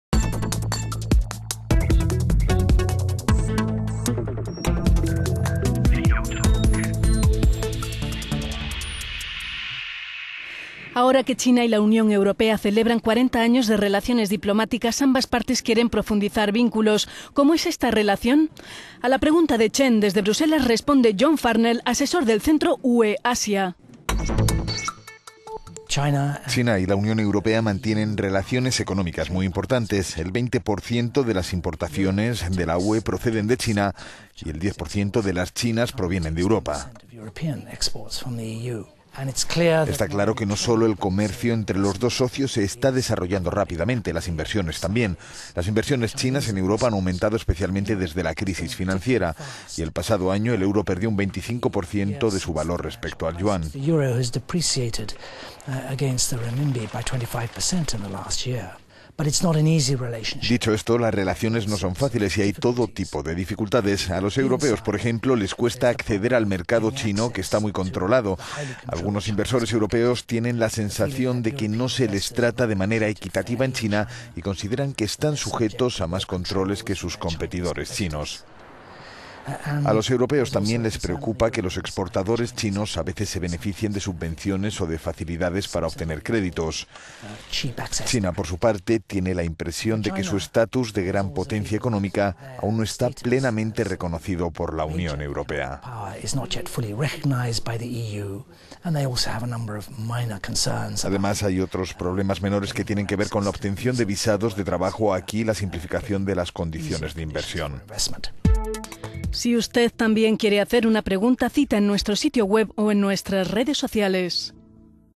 Relaciones comerciales entre China y la UE - utalk
"Ahora que China y la UE celebran 40 años de relaciones diplomáticas, ambas partes quieren profundizar vínculos. ¿Cómo es esta relación?" Cheng, Bruxelles."

A la pregunta de Cheng desde Bruselas responde John Farnell, asesor del Centro UE-Asia.

John Farnell, Senior Adviser, EU-Asia centre

"China y la UE mantienen relaciones económicas muy importantes. El 20% de las importaciones de la UE proceden de China, y el 10% de las importaciones chinas provienen de Europa.